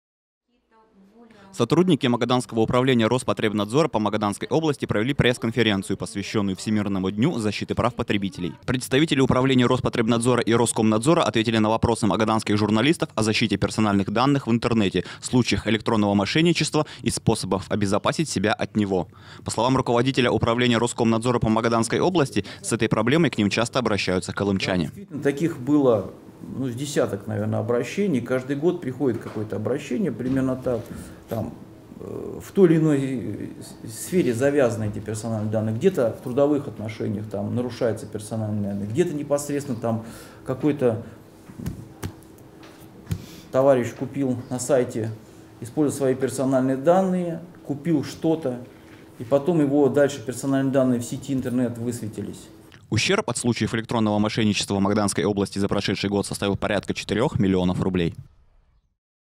Пресс-конференция, посвященная Всемирному дню защиты прав потребителей в Роспотребнадзоре
Сотрудники управления Роспотребнадзора по Магаданской области провели пресс-конференцию, посвященную Всемирному дню защиты прав потребителей. В этом году он проходит под девизом: «Цифровой мир: надежные смарт-устройства». Представители управлений Роспотребнадзора и Роскомнадзора ответили на вопросы Магаданских журналистов о защите персональных данных в интернете, случаях «электронного мошенничества» и способах обезопасить себя от него. По словам руководителя управления Роскомнадзора по Магаданской области, с этой проблемой к ним часто обращаются Колымчане.